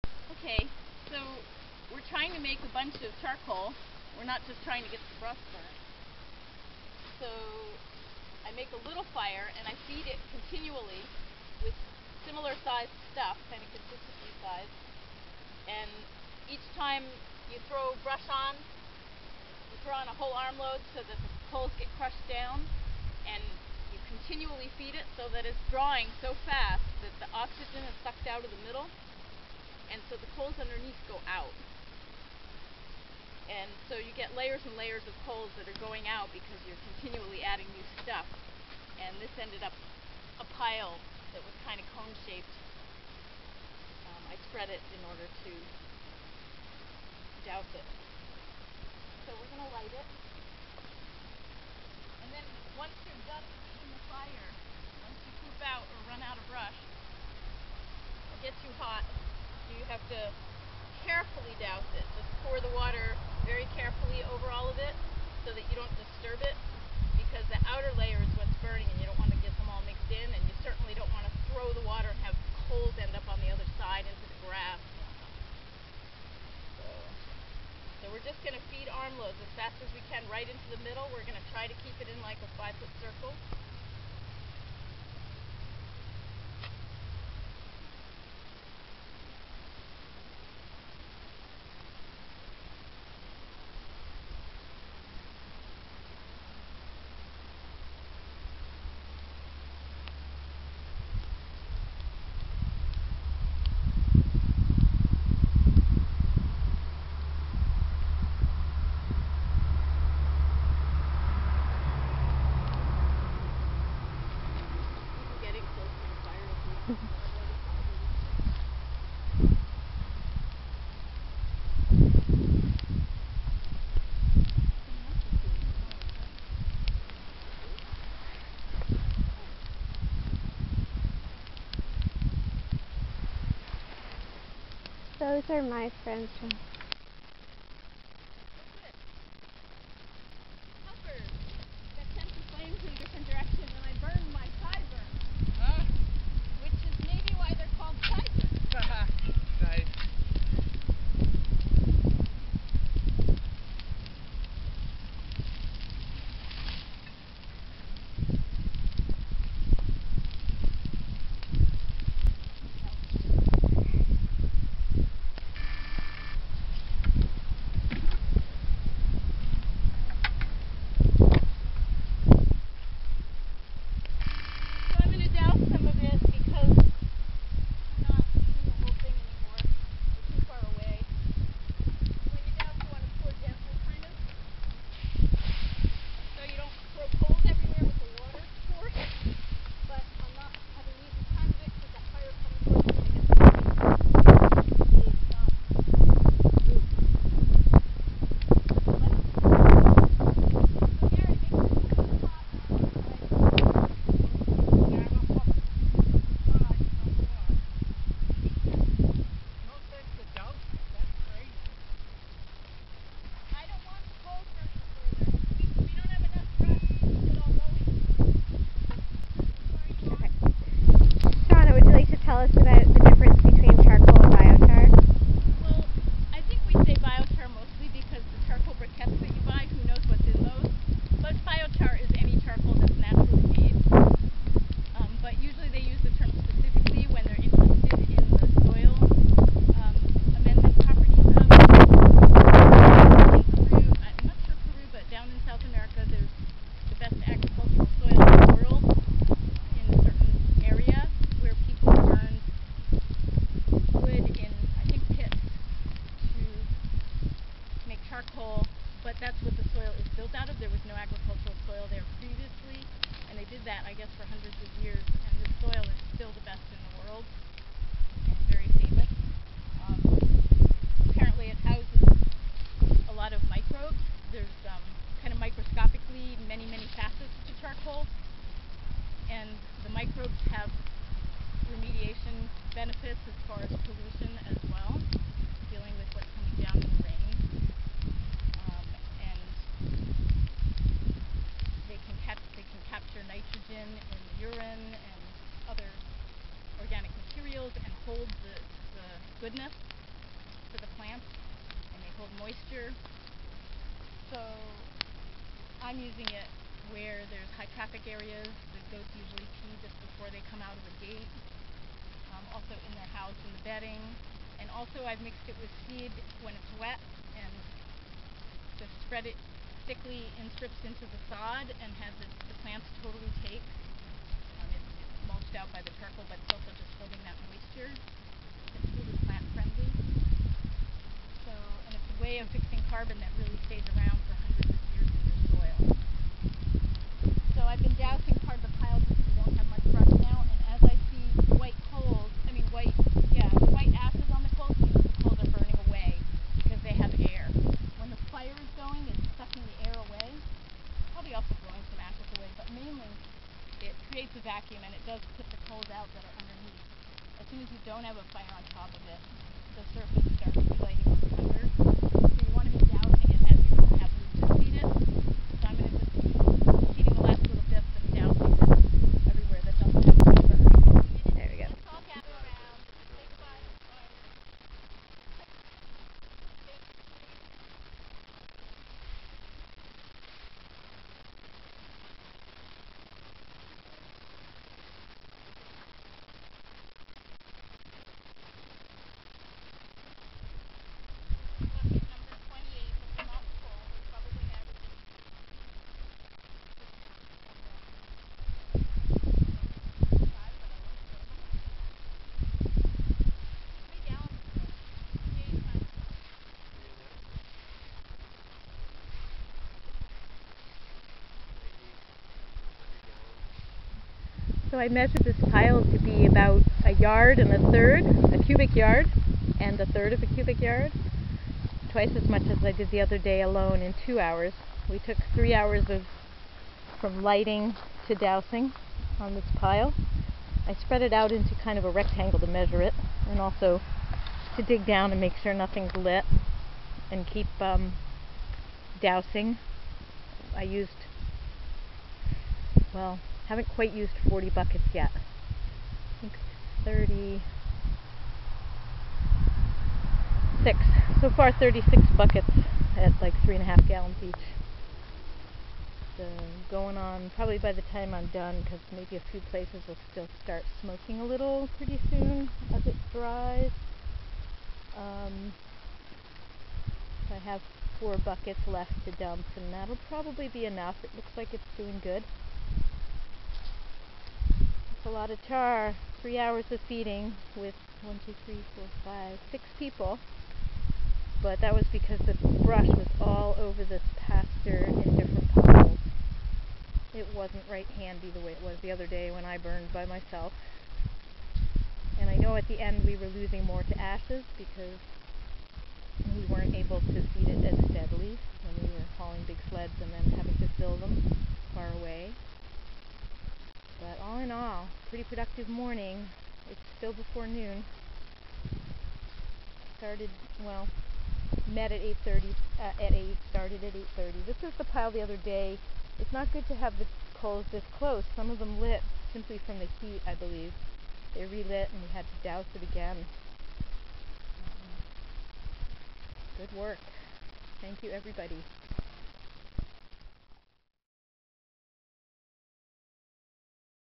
Simple Method for Making Biochar
Shana's method for making biochar at 3 Streams Farm.

Want to learn more about pollarding and making tree leaf fodder for livestock?